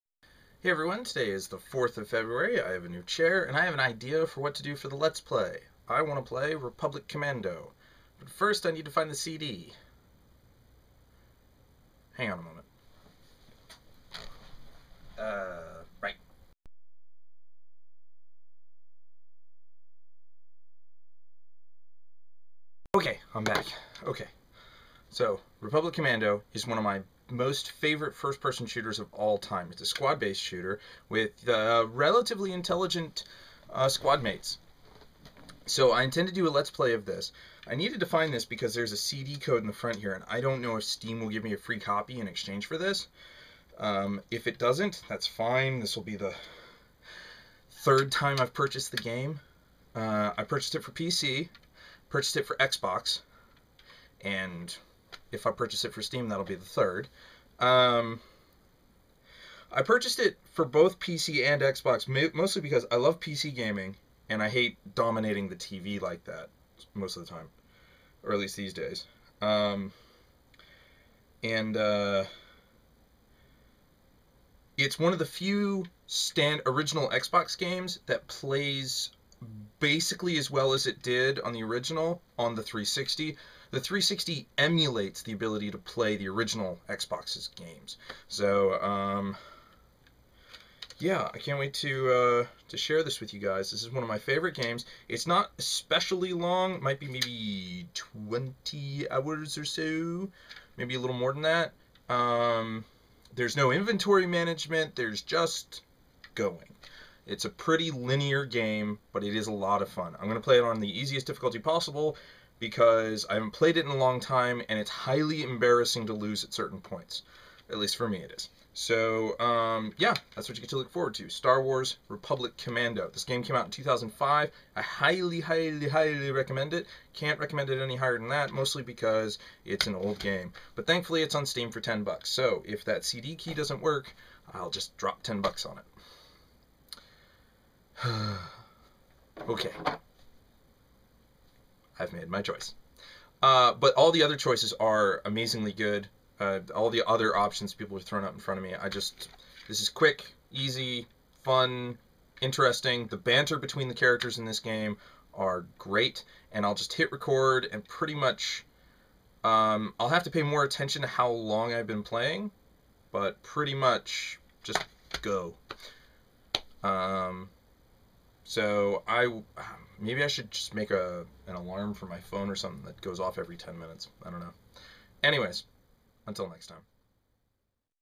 I've Made A Choice
In which I announce my choice for my next Let's Play: Republic Commando. One of my most favorite FPS games of all time. Oh, the CD code didn't work. Alas.